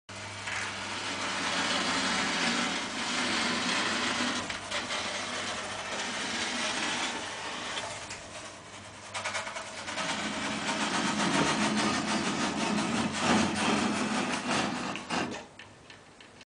defect poort feryn
houder bovenste rol schuurd tegen bovenste rail